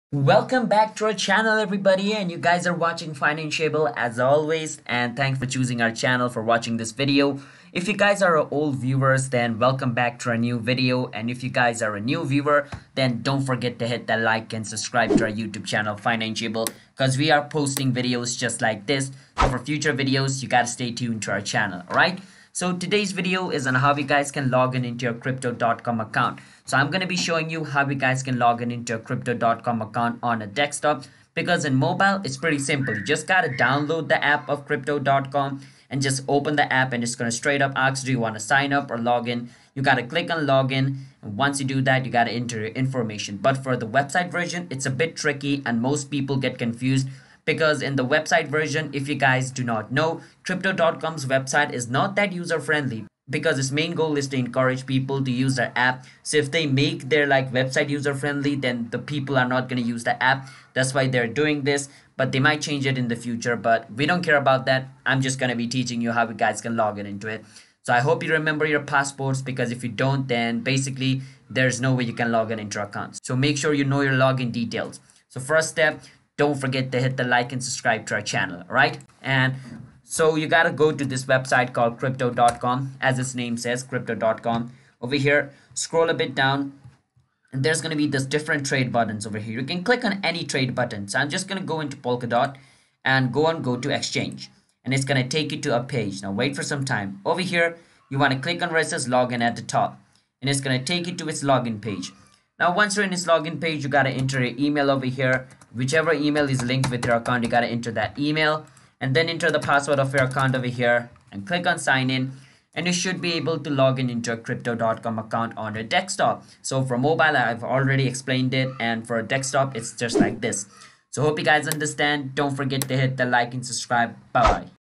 How to Login to Crypto.com | Crypto.com Login, Sign In (2022)
Learn the exact steps on How to Login to Crypto.com. Make sure you watch the video till the end for better assistance.

CHAPTERS
0:00 - Intro
0:26 - How to Login to Crypto.com
2:00 - Outro